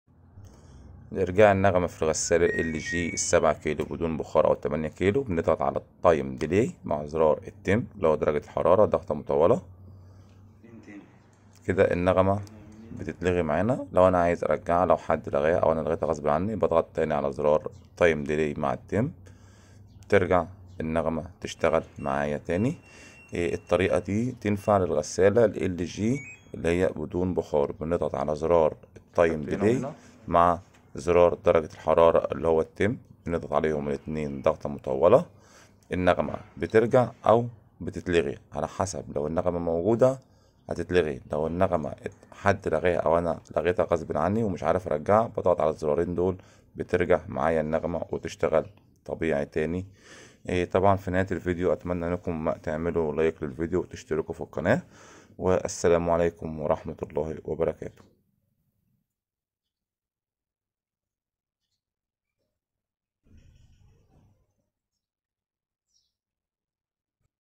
استعادة صوت التنبيه غسالة LG اتوماتيك بدون بخار
كيفية استرجاع صوت نغمة الغسالة ال جى 7 كيلو 8 كيلو 9 كيلو بدون بخار